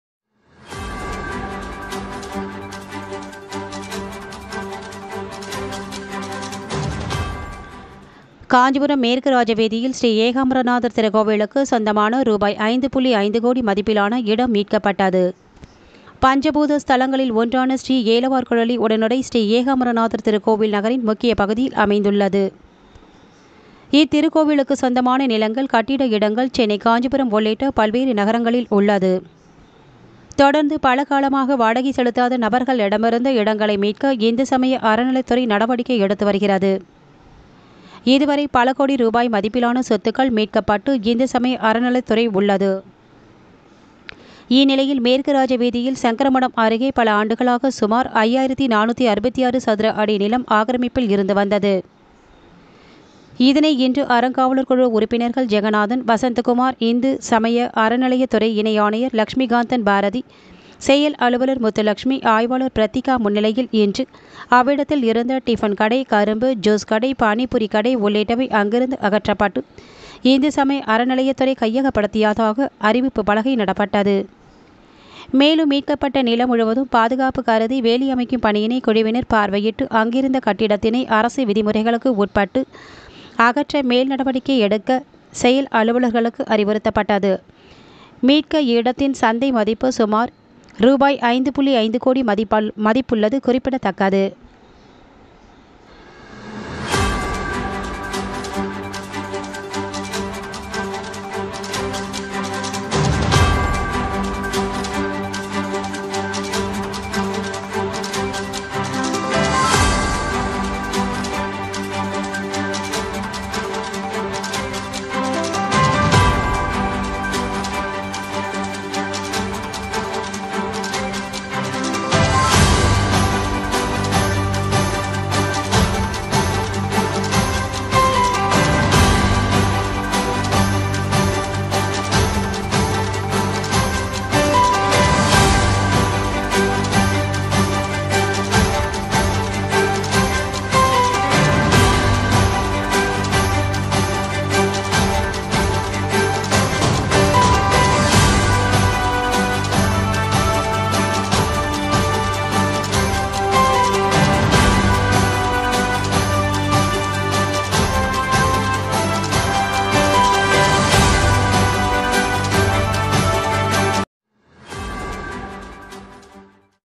ஸ்ரீ ஏகாம்பரநாதர் திருக்கோவிலுக்கு சொந்தமான 5.5 கோடி மதிப்பில் ஆன இடம் மீட்கப்பட்டது
காஞ்சிபுரம் மேற்கு ராஜ வீதியில் , ஸ்ரீ ஏகாம்பரநாதர் திருக்கோவிலுக்கு சொந்தமான ₹5.5 கோடி மதிப்பிலான இடம் மீட்கப்பட்டது.

பஞ்சபூத தலங்களில் ஒன்றான ஸ்ரீ ஏலவார்குழலி உடனுறை ஶ்ரீ ஏகாம்பரநாதர் திருக்கோயில் நகரின் முக்கிய  பகுதியில் அமைந்துள்ளது .

இத்திரு கோவிலுக்கு சொந்தமான நிலங்கள் , கட்டிட இடங்கள் சென்னை காஞ்சிபுரம் உள்ளிட்ட பல்வேறு நகரங்களில் உள்ளது. தொடர்ந்து பல காலமாக வாடகை செலுத்தாத நபர்களிடமிருந்து இடங்களை மீட்க இந்து சமய அறநிலைத்துறை நடவடிக்கை எடுத்து வருகிறது.

இதுவரை பல கோடி ரூபாய் மதிப்பிலான சொத்துக்கள் மீட்கப்பட்டு இந்து சமய அறநிலைத்துறை உள்ளது.

இந்நிலையில் மேற்கு ராஜ வீதியில் சங்கரமடம் அருகே பல ஆண்டுகளாக சுமார் 5, 466 சதுர அடி நிலம் ஆக்கிரமிப்பில் இருந்து வந்தது.

இதனை இன்று அறங்காவலர் குழு உறுப்பினர்கள் ஜெகநாதன், வசந்தா சுகுமார் , இந்து சமய அறநிலையத்துறை இனைஆணையர் லட்சுமி காந்தன் பாரதி, செயல் அலுவலர் முத்துலட்சுமி, ஆய்வாளர் பிரித்திகா  முன்னிலையில் இன்று அவ்விடத்தில் இருந்த டிபன் கடை,  கரும்பு ஜீன்ஸ் கடை, பானிபூரி கடை உள்ளிட்டவை அங்கிருந்து அகற்றப்பட்டு இந்து சமய அறநிலையத்துறை கையகப் படுத்தியதாக அறிவிப்பு பலகை நடப்பட்டது.

மேலும் மீட்கப்பட்ட நிலம் முழுவதும் பாதுகாப்பு கருதி வேலி அமைக்கும் பணியினை குழுவினர் பார்வையிட்டு அங்கிருந்த கட்டிடத்தினை அரசு விதிமுறைகளுக்கு உட்பட்டு அகற்ற மேல் நடவடிக்கை எடுக்க செயல் அலுவலர்க்கு அறிவுறுத்தப்பட்டது.

மீட்க இடத்தின் சந்தை மதிப்பு சுமார் ரூ 5.5 கோடி மதிப்பள்ளது கூறிபிடதக்கது.